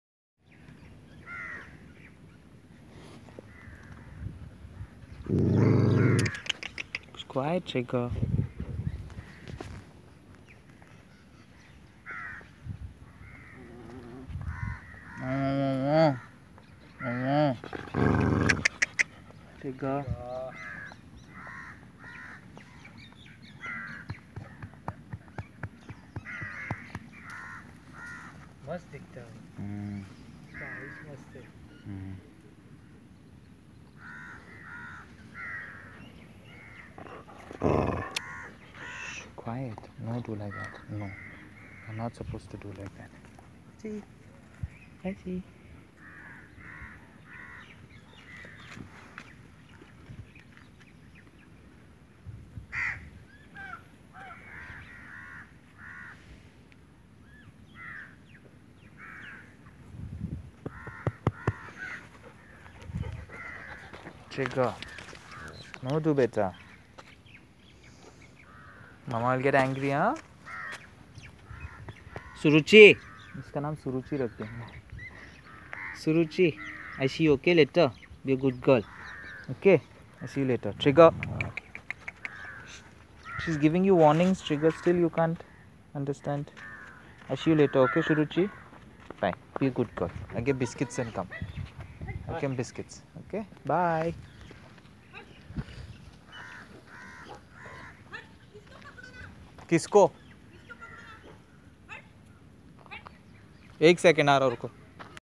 Introducing Suruchi the matriarch packleader at the beach
packlife and all the puppies allow us to enter their territory since we pose no harm. We come in peace.